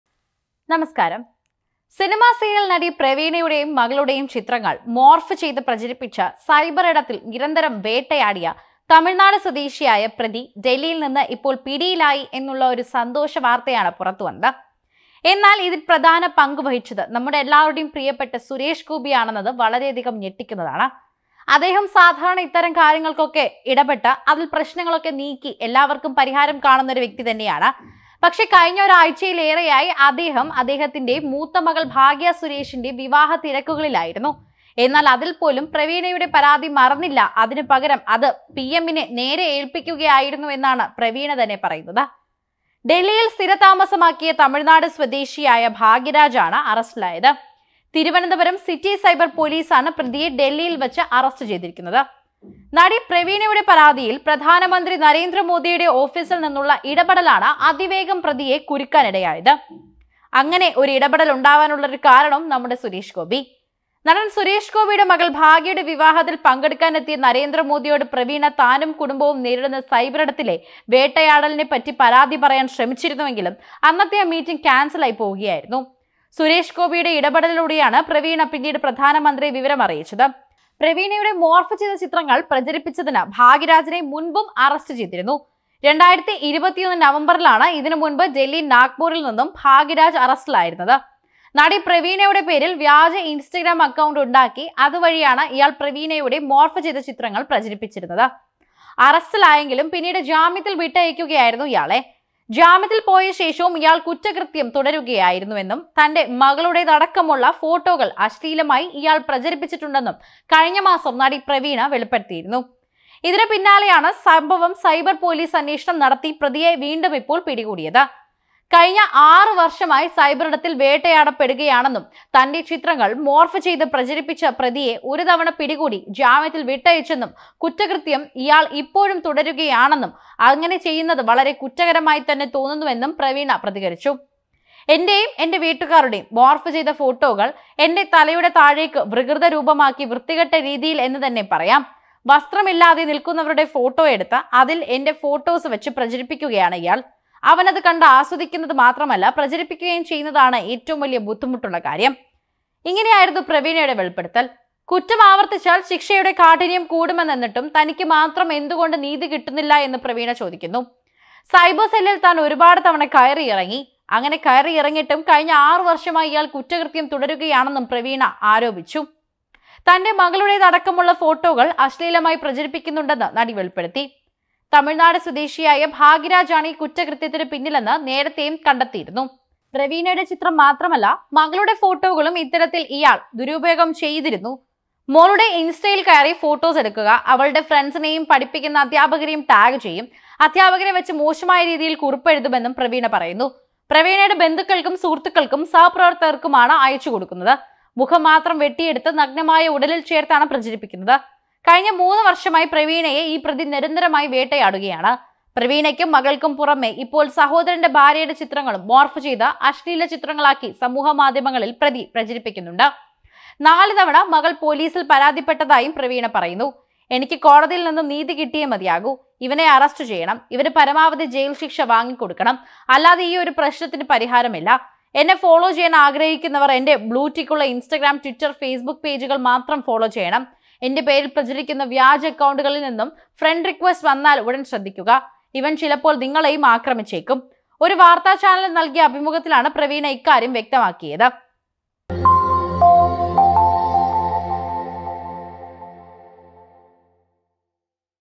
സുരേഷ് ഗോപി ഇടപെട്ടു.. പ്രവീണയ്ക്ക് സമാധാനം..! | actress Praveena | Suresh Gopi
പ്രവീണയെ നാറ്റിച്ചവന്
എട്ടിൻ്റെ പണി കൊടുത്ത് സുരേഷ് ഗോപി...